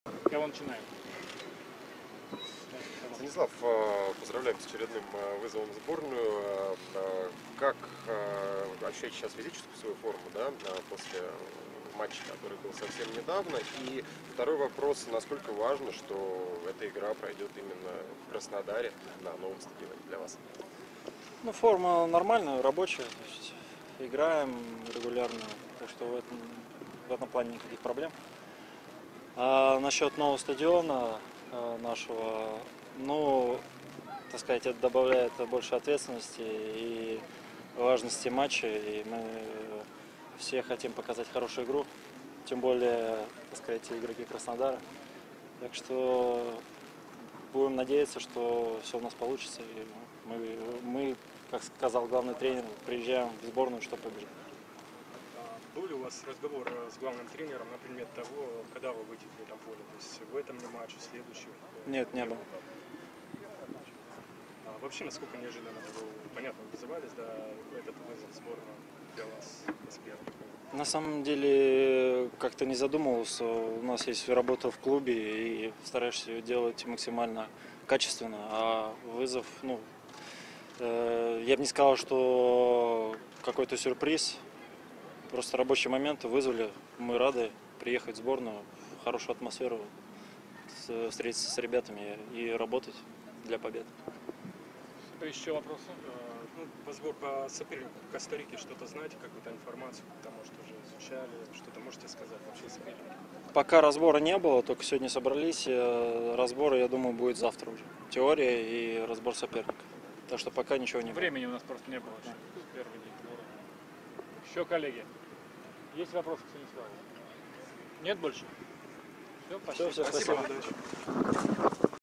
Станислав Крицюк: игра на новом стадионе добавляет ответственности
Вратарь "Краснодара" Станислав Крицюк поделился ожиданиями от предстоящего контрольного матча с Коста-Рикой.

- Игра на новом стадионе "Краснодара" добавляет ответственности, важности матча. Будем надеяться, что все у нас получится. Как сказал главный тренер, мы приезжаем в сборную, чтобы победить.

- Был ли разговор с главным тренером на предмет того, когда вы выйдете на поле?

- Нет, не было.

- На сколько неожиданным был вызов в сборную?

- На самом деле как-то не задумывался. У нас есть работа в клубе, и стараюсь делать ее максимально качественно. Я бы не сказал, что это какой-то сюрприз. Рабочий момент. Рады приехать в сборную, хорошая атмосфера. Будем играть на победу.

- Что можете сказать о Коста-Рике?

- Пока разбора соперника не было, мы ведь только собрались. Разбор и теория будет завтра.